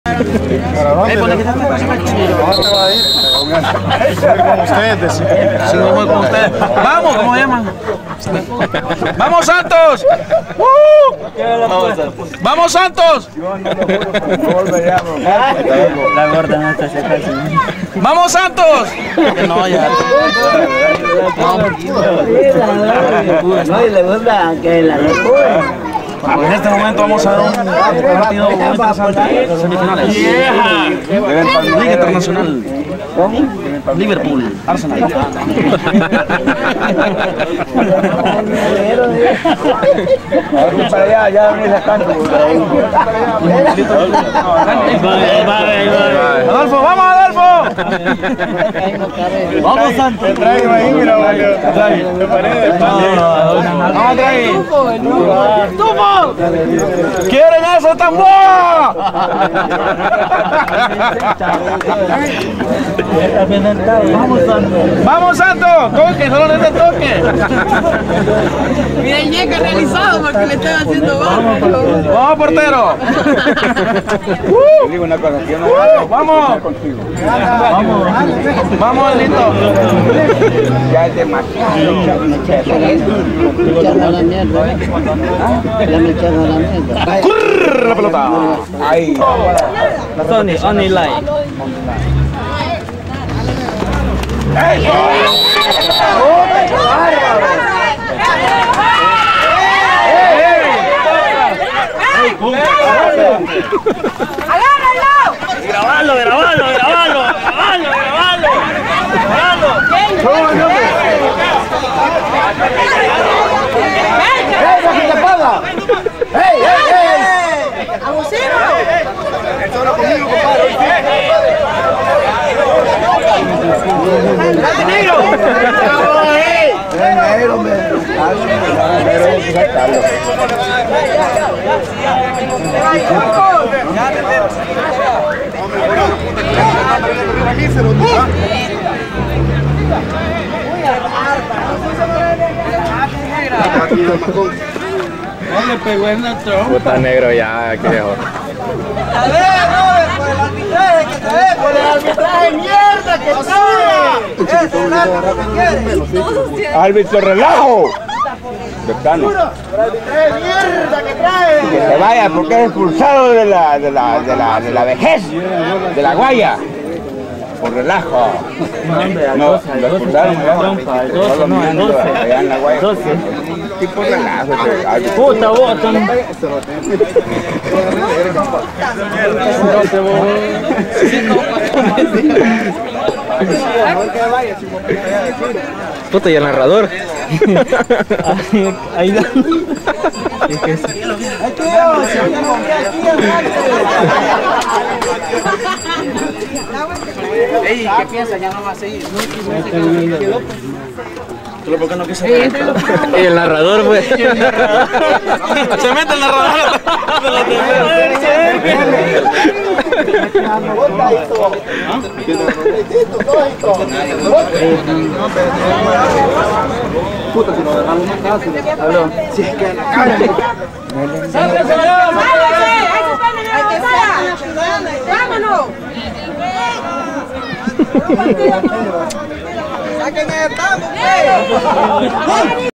DESBERGUE 2010.mp4
2 do partido de las semifinales entre el DC7 y el SANTOS el primer partido lo gano el Santos 2-1..

Saludos